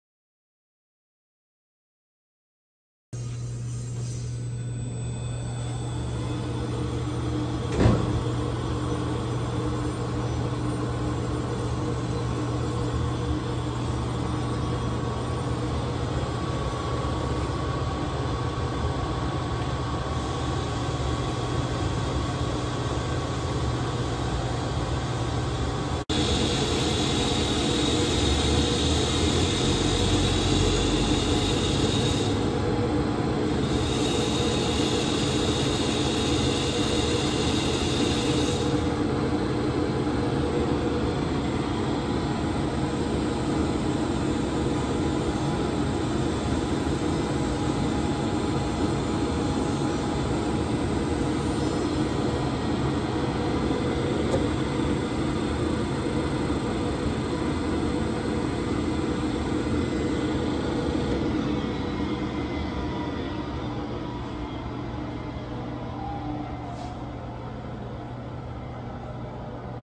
Granutec Pull Roll Granulator - Stock Number19279A
Used 12" x 12" Granutec TFG1212 Pull Roll Granulator
Three Knife Open Rotor
Two Bed Knives
DC Pull Roll System With Drive
Includes Blower With AC Variable Speed Drive
On Casters
S/N 4962781

Approximate Dimensions: 34" L x 30" W x 52"